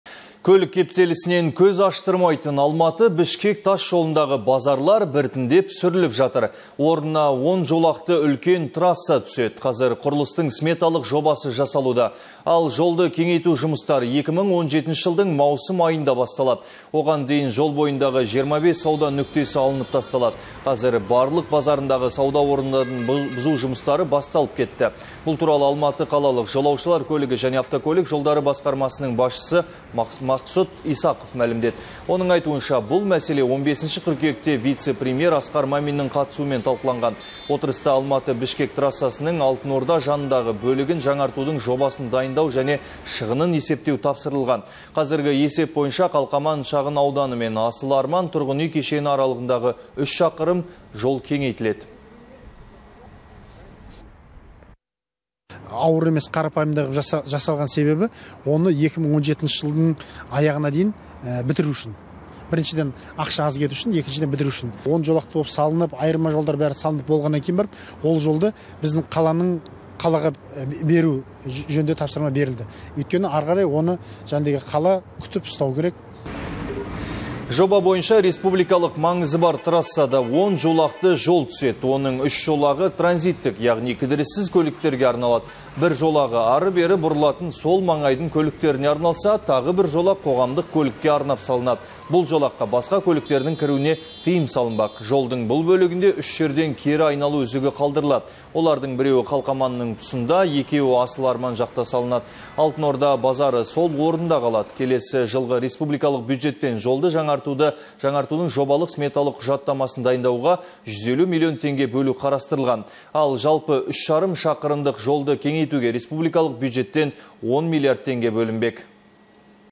Алматы-Бішкек тас жолындағы базарлар біртіндеп сүрілуде (27.09.16)
Орнына он жолақты үлкен трасса түседі. Қазір құрылыстың сметалық жобасы жасалуда. Ал, жолды кеңейту жұмыстары 2017 жылдың маусым айында басталады.